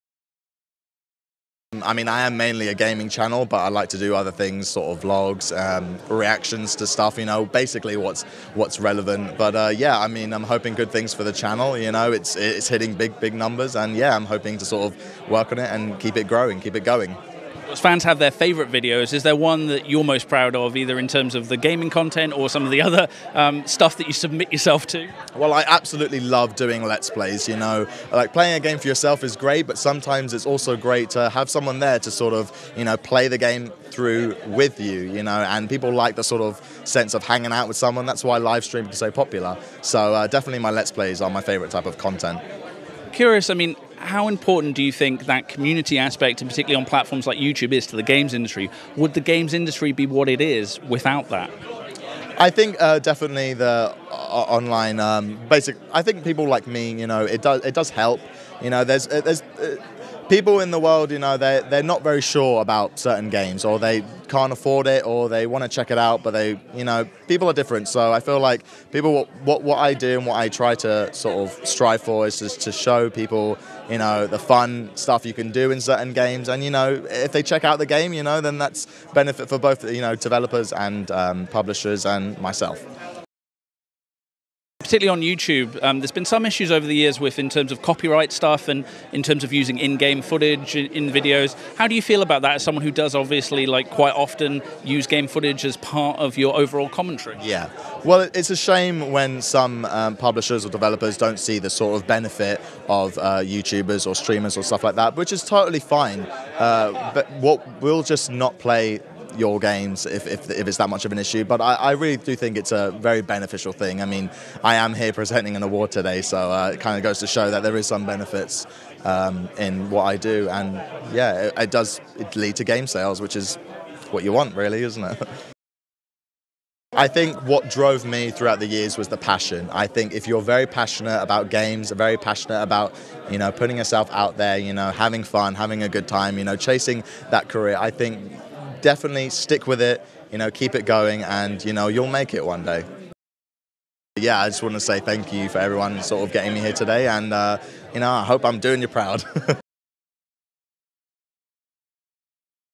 MessYourself Interview YouTube & Gaming
Red Carpet News TV talks to VIP guests, nominees and winners on the red carpet and backstage at the British Academy Games Awards 2018 in London.